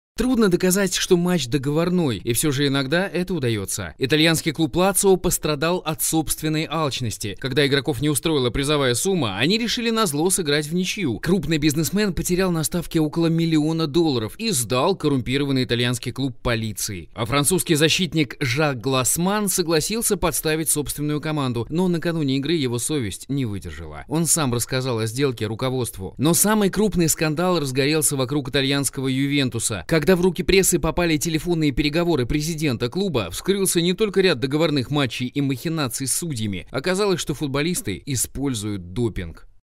Инфографика. Жадностъ сгубила футболиста.
20/11/2010
Инфографика
Жадностъ сгубила футболиста

Трудно доказать, что матч договорной. И всё же иногда это удаётся. Итальянский клуб "Лацио" пострадал от собственной алчности. Когда игроков не устроила призовая сумма, они решили назло сыграть вничью. Крупный бизнесмен потерял на ставке около миллиона долларов и сдал коррумпированный итальянский клуб полиции. А французский защитник Жак Глассманн согласился подставить собственную команду, но накануне игры его совесть не выдержала. Он сам рассказал о сделке руководству. Но самый крупный скандал разгорелся вокруг итальянского "Ювентуса". Когда в руки прессы попали телефонные переговоры президента клуба, вскрылся не только ряд договорных матчей и махинаций с судьями. Оказалось, что футболисты используют допинг.